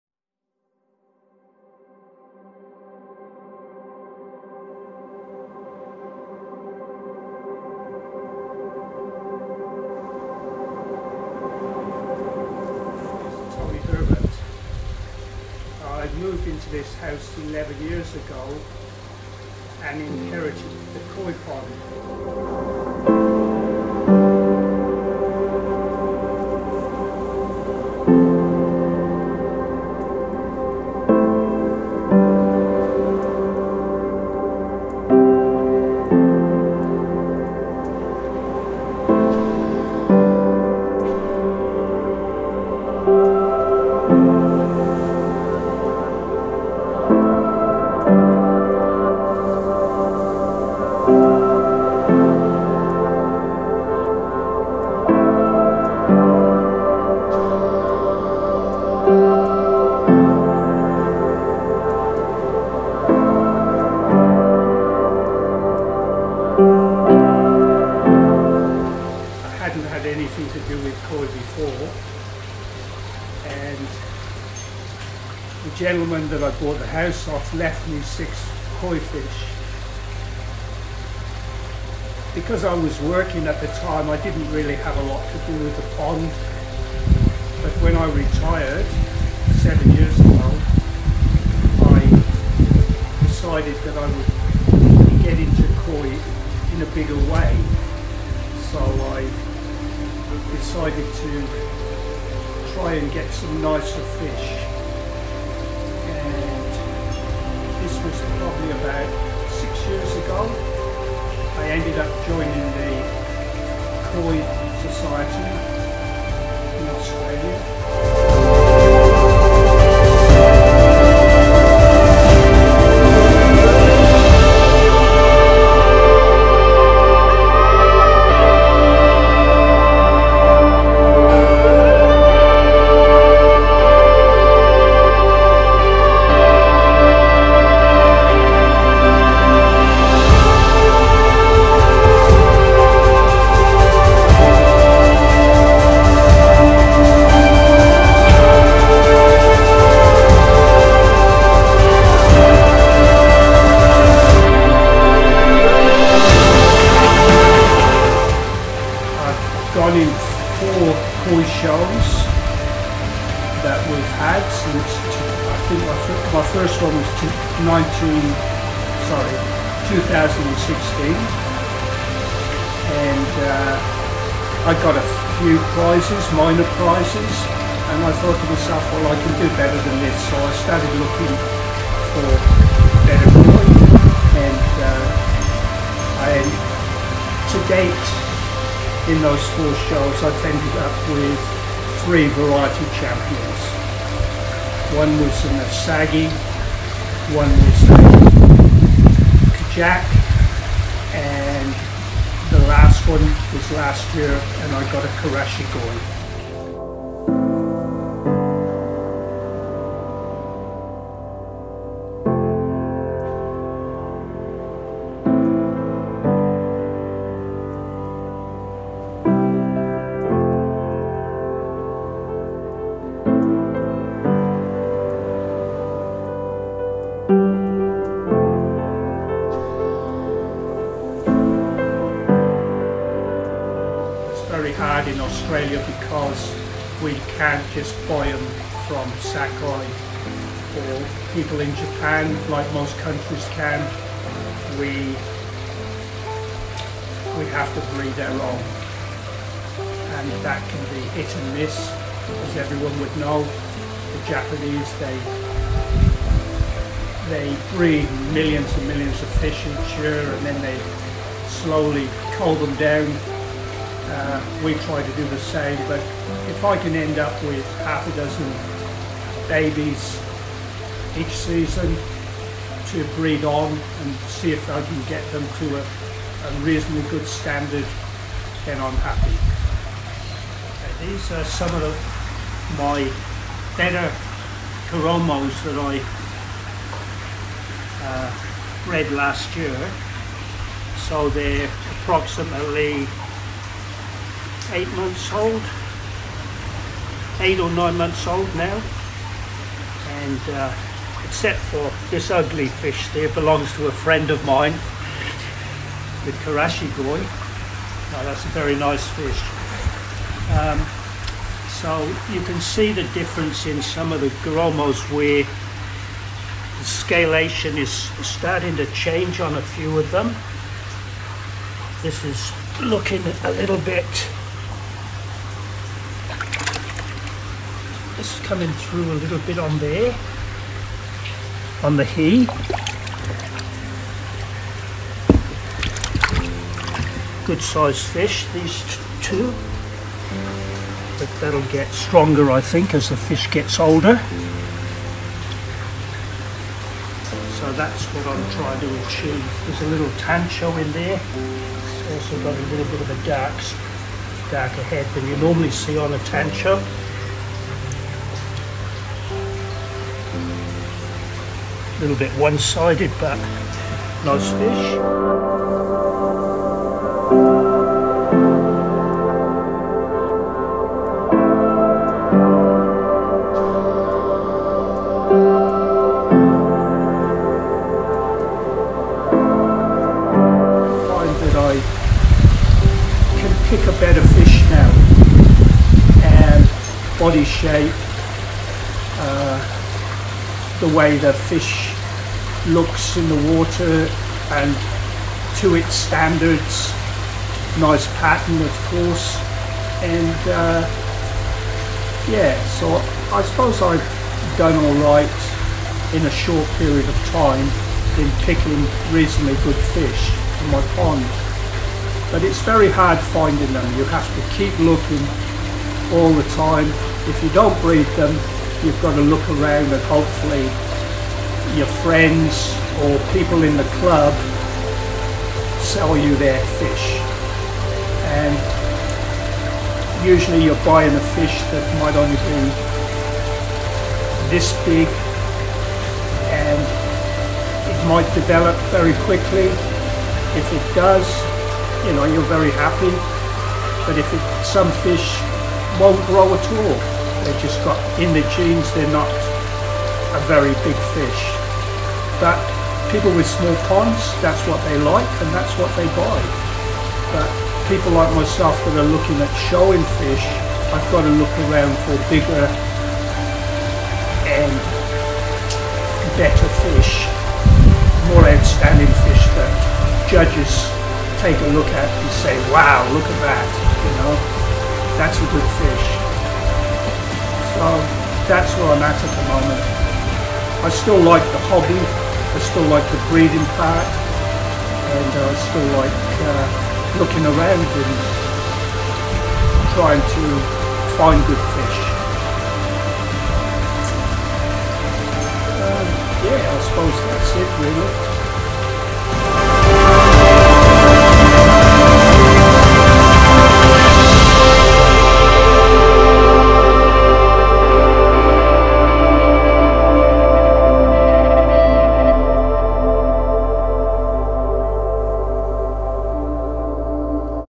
Profile of a Koi Hobbyist - Tony Herbert
This video marks the debut of 'The Koi Journey'.

Follow along as we hear Mr Tony Herbert of Western Australia share his koi journey with us.
Thank you for making this dream of mine happen, Tony.
I greatly appreciate that.

This would mean a huge lot in my pursuit of the highest quality content I can provide you viewers with.